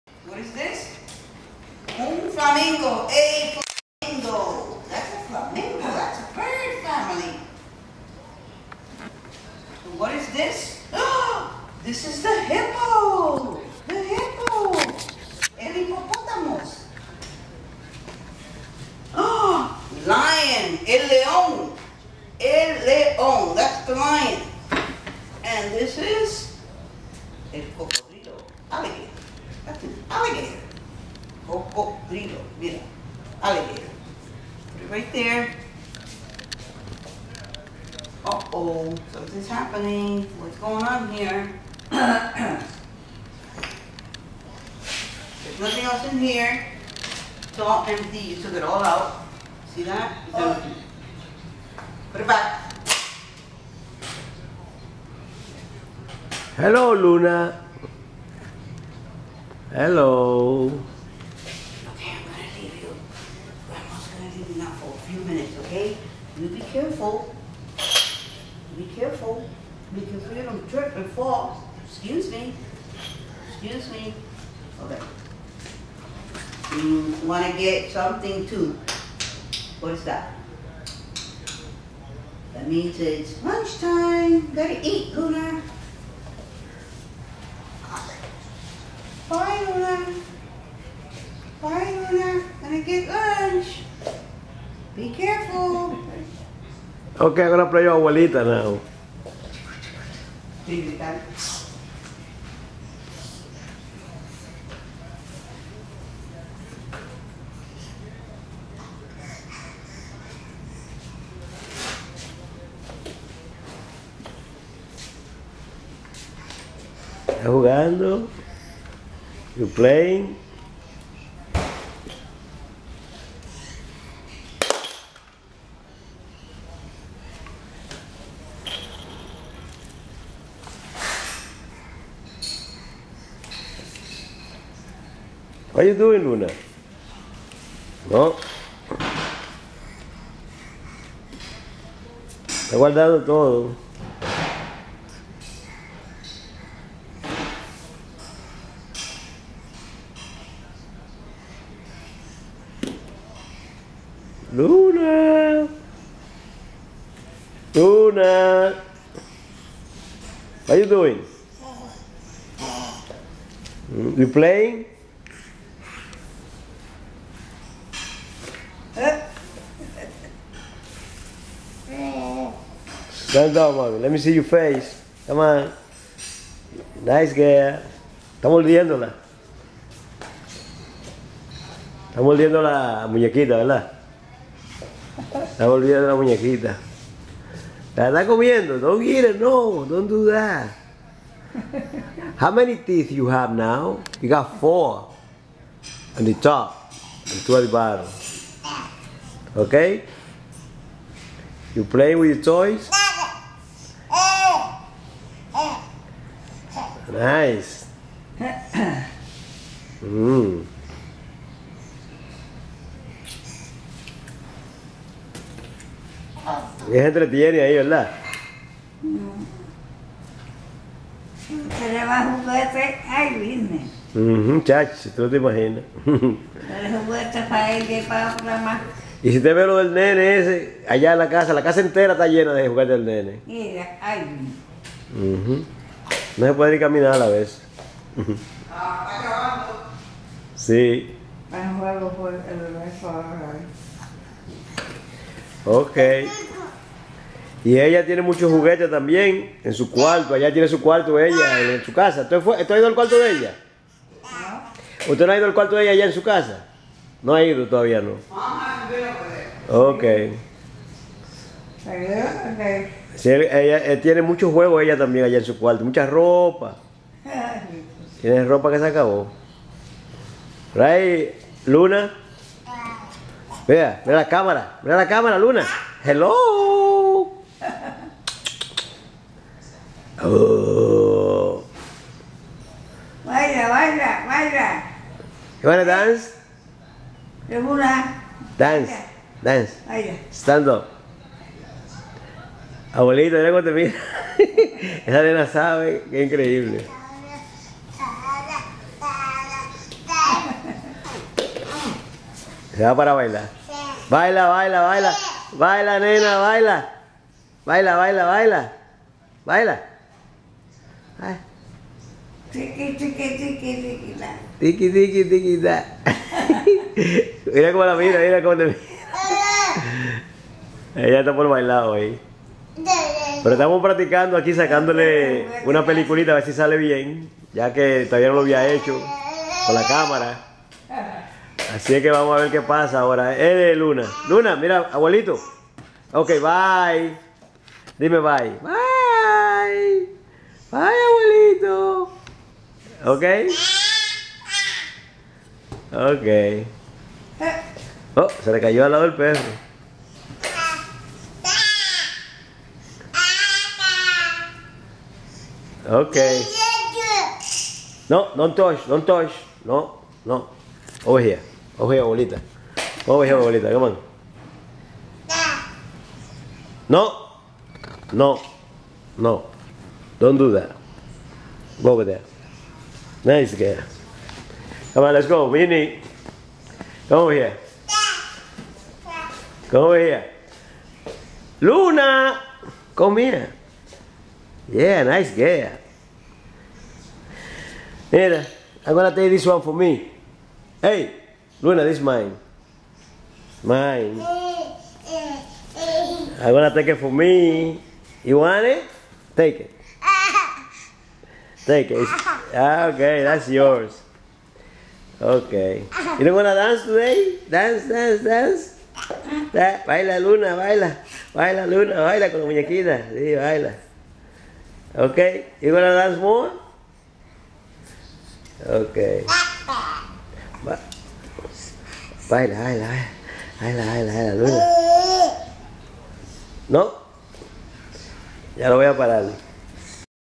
MVI 0003 Luna playing and moving 09-30-18
less than two weeks before her first year. she has 4 teeth on top and 2 at the bottom.